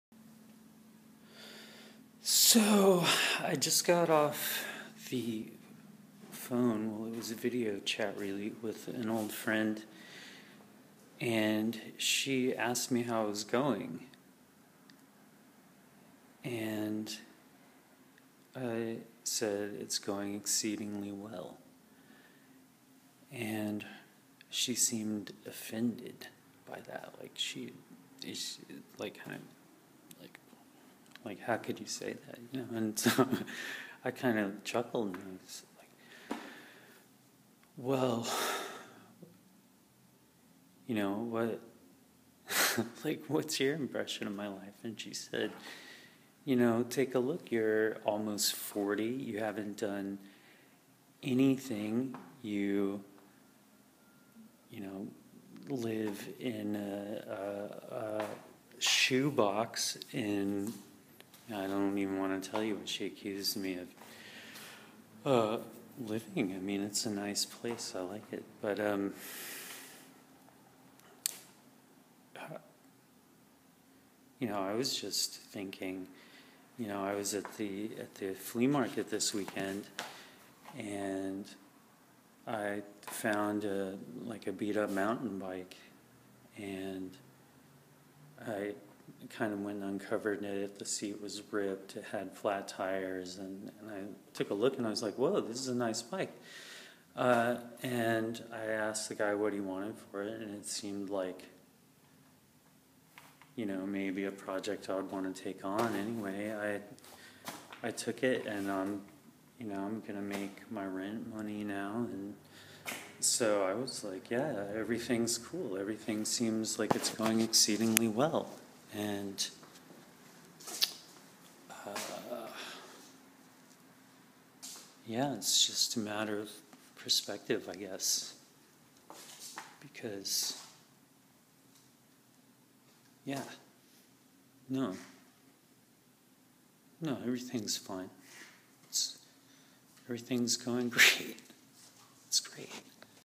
Follow up to question: how's it going? Minutes Phone AKA Talk Time Diary segment in paintings s
Reed talks to camera like future Oprah Winfrey guest during what will later be referred back to as one of the difficult times. Season one.